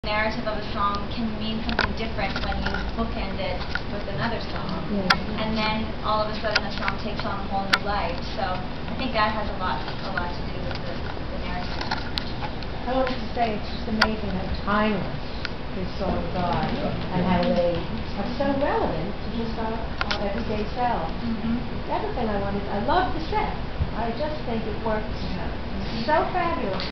Jacques Brel Talkback with constantine maroulis
A Jacques Brel talkback with the cast on Jan. 15th...